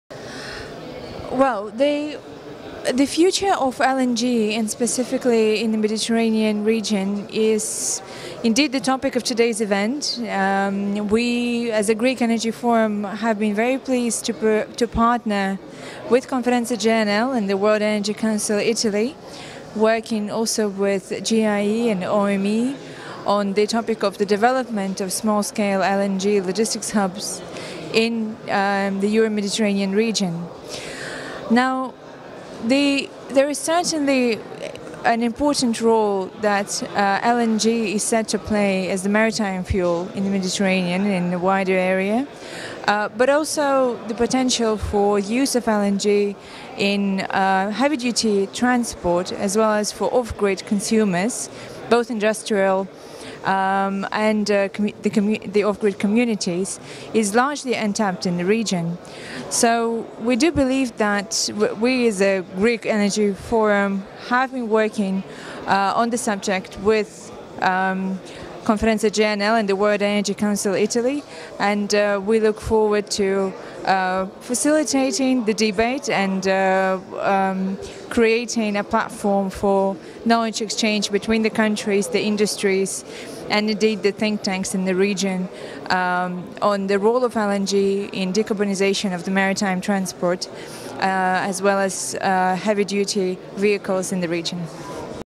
ConferenzaGNL 2019 | Interviews | Daria Nochevnik, Greek Energy Forum
Daria Nochevnik, Deputy Head, Greek Energy Forum | The SSLNG Use, Euro-Mediterranean Conference&Expo | 15/16 May 2019